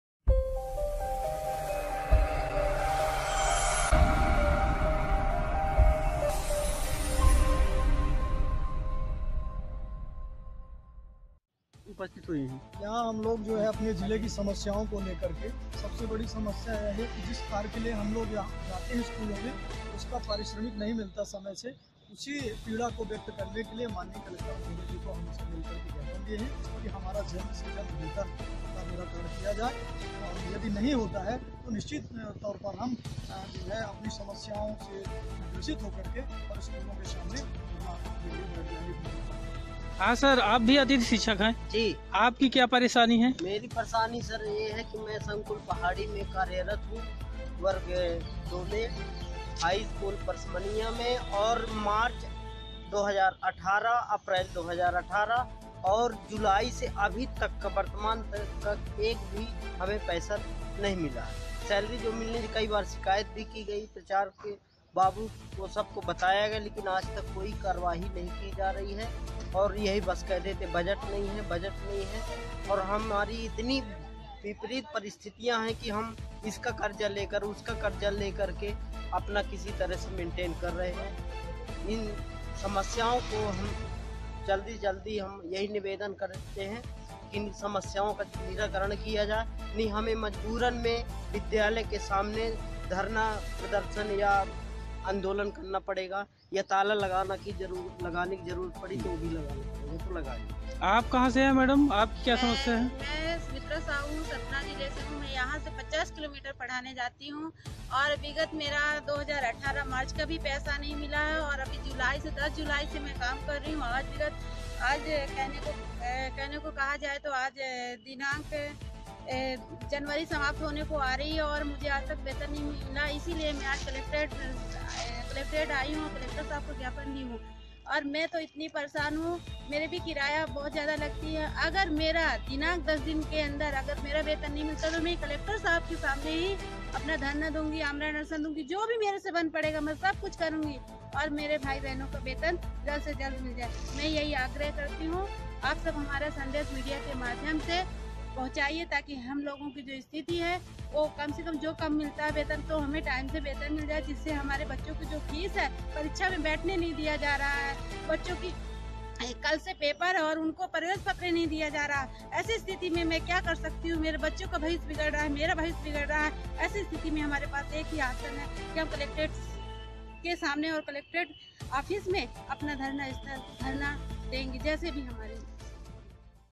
Atithi shikshak latest news | अतिथि शिक्षकों के वेतन अहरण के लिए सबसे बडी खबर
Mp tak
(म.प्र) उच्च मध्यमिक शिक्षक भर्ती काउंसलिंग वर्ग-1
(म.प्र)  मध्यमिक शिक्षक भर्ती काउंसलिंग वर्ग-2